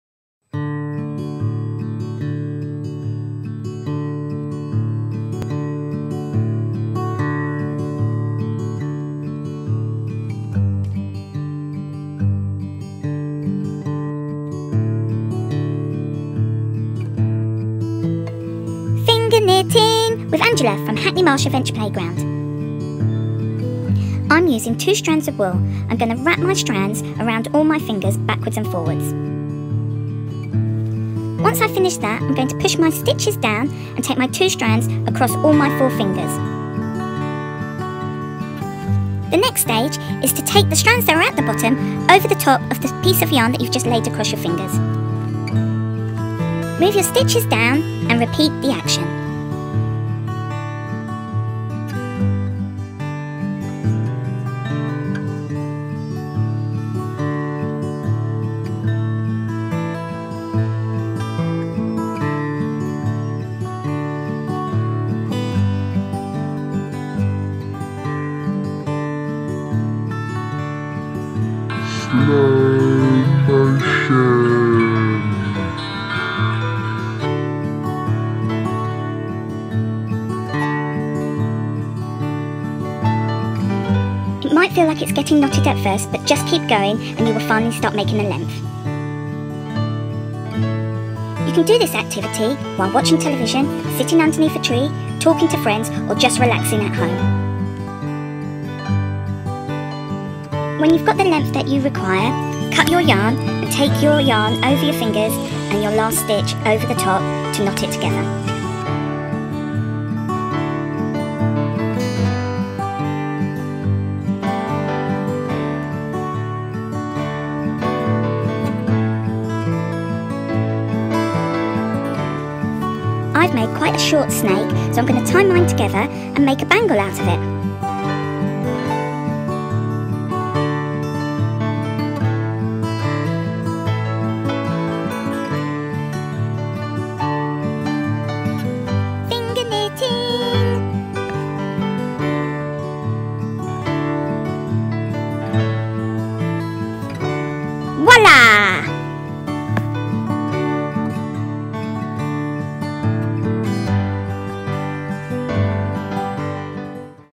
Learn how to finger knit!
Join our play team at Hackney Marsh Adventure Playground and learn how to finger knit! Super fun and super easy once you get the hang of it!